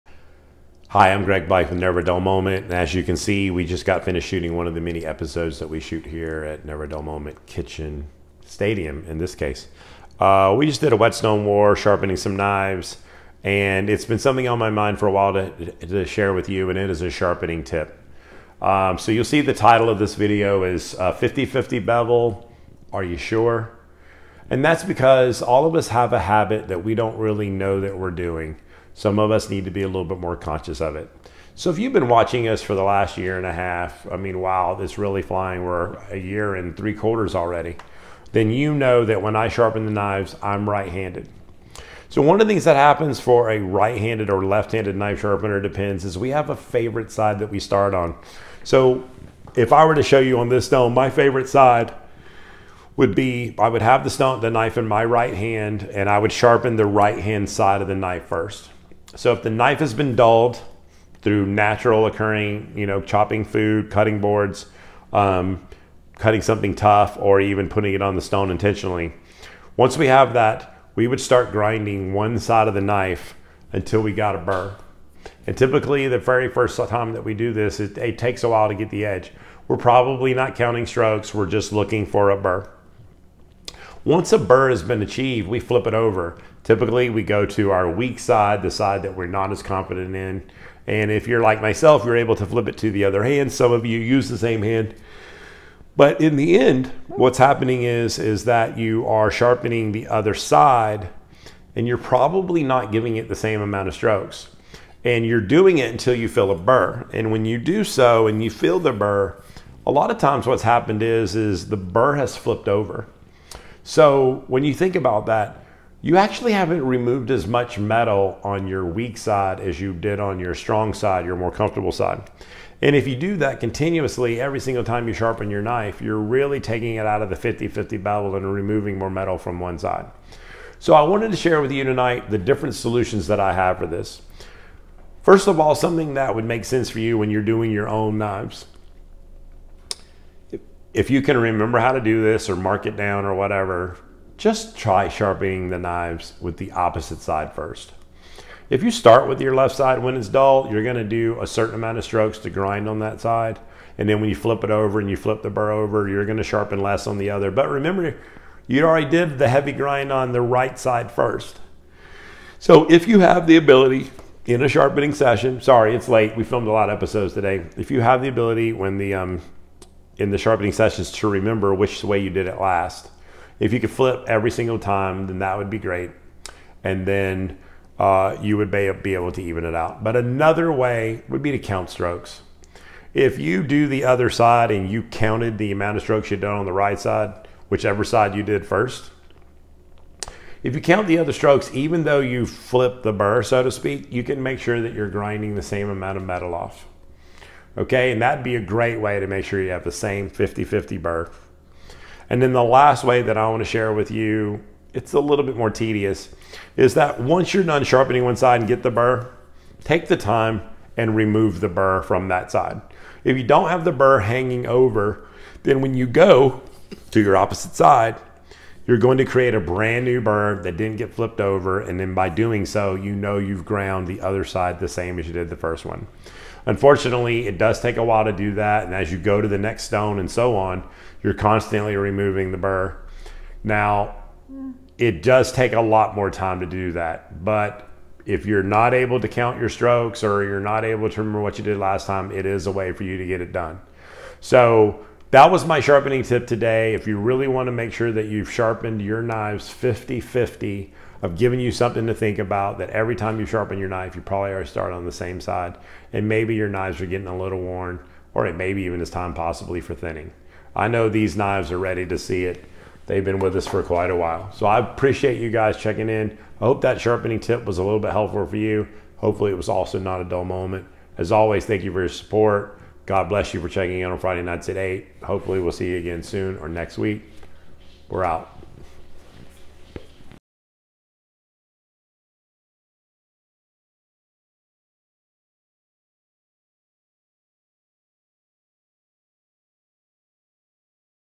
Knife Knowledge/Knife Basics: 50/50 Bevel - Are you sure? How can you be sure?
Today we discuss the possibility that you have not kept your bevel even. A lot of us have bad habits. It's easy to get away from us. Sharpening your knife can be taken for granted. You can lose track and not sharpened one side as evenly as the other. This is not a problem at first. It becomes a problem overtime. The profile of your knife may have changed dramatically. These are a few steps or how you can make sure that you have, once again, made your edge even. Sharpening on a whetstone is only as precise as the person doing it.